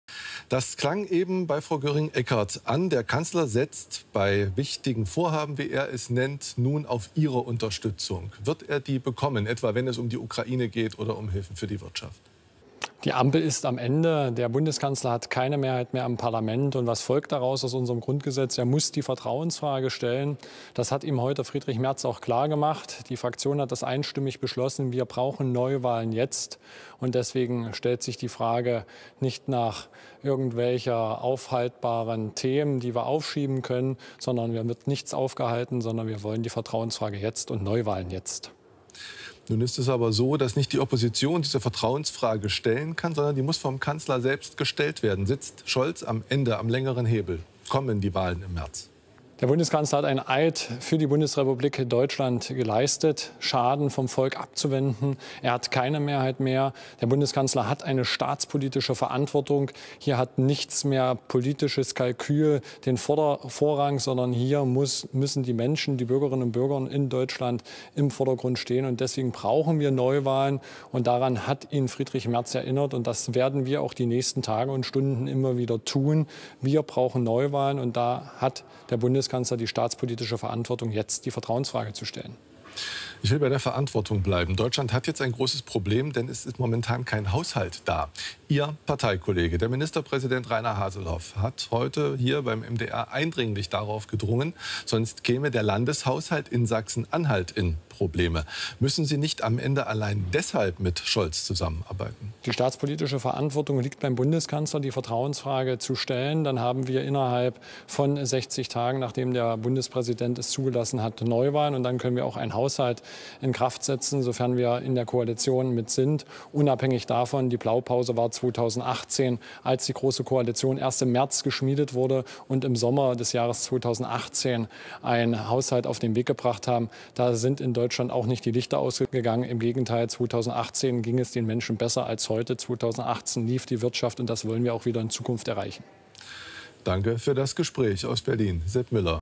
VERTRAUENSFRAGE JETZT - NEUWAHLEN JETZT
Das Volk sollte schnellstmöglich entscheiden. Es braucht zwingend Neuwahlen- Jetzt! Die 🟥🟩-Übergangsregierung muss beendet werden. Die Herausforderungen sind viel zu groß. Wir können uns keine Hängepartie leisten.